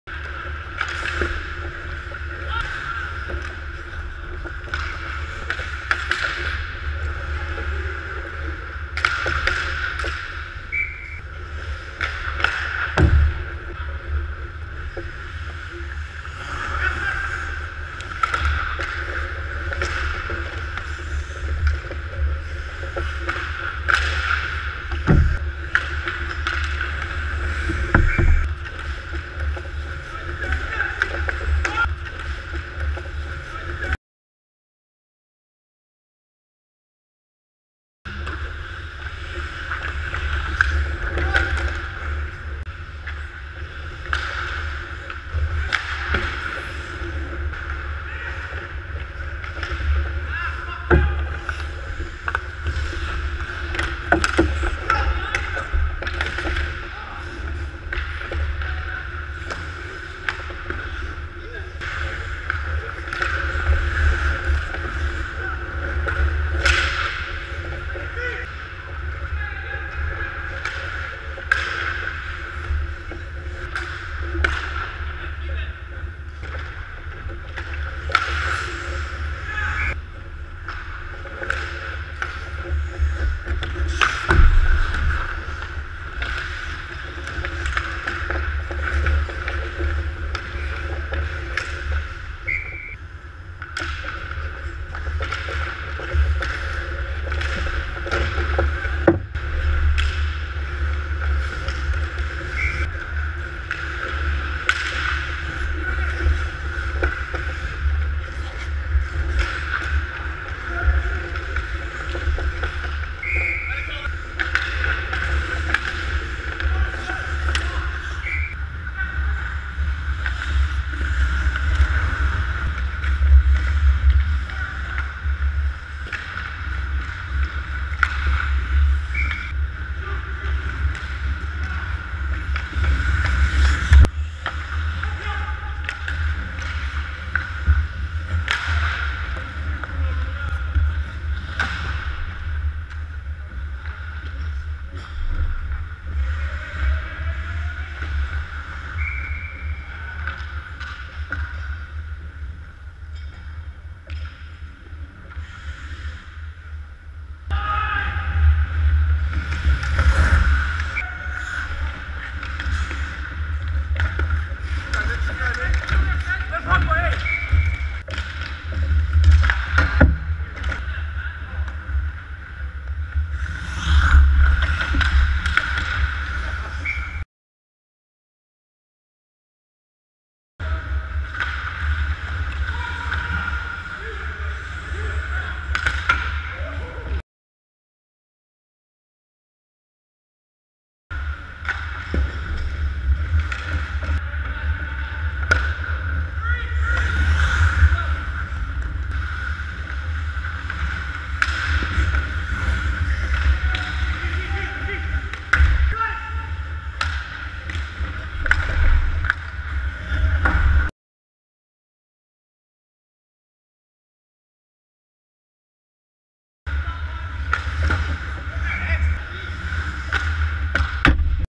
January 9th 2014, Div 4 GoPro
Camera wonked out on me again... stopped filming at the start of the 3rd. 1 more goal against in a 3-2 loss.